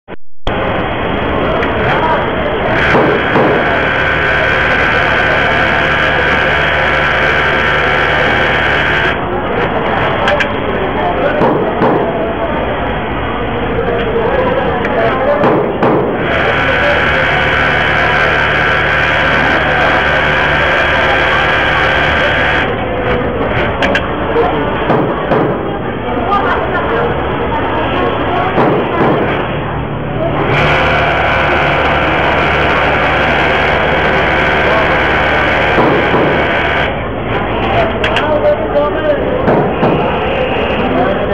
KANSAI SPECIAL RX9803ALK-UTC1 | 3 NEEDLE SEMI-CYLINDER BED FLATLOCK FOR BOTTOM HEMMING
Kansai Special, 3 needle semi-cylinder bed flatlock for bottom hemming with under-thread cutter.